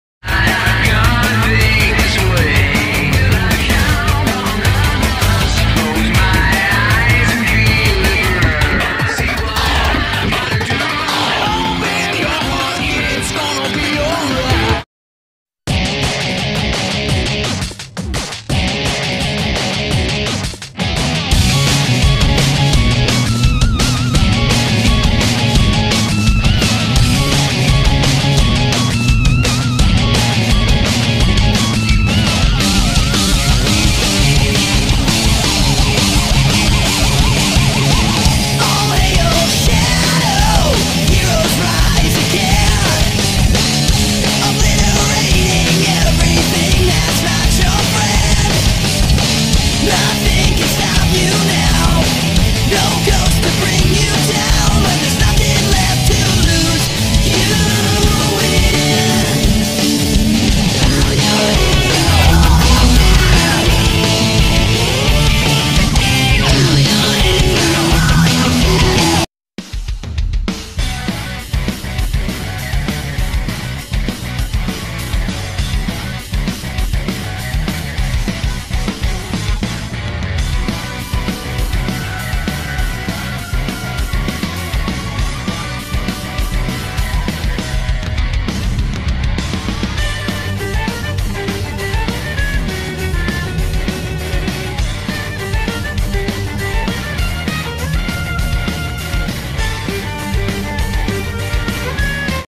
Dark Thomas gets turned into a baby
While preparing for a day out something unexpected happens to Dark Thomas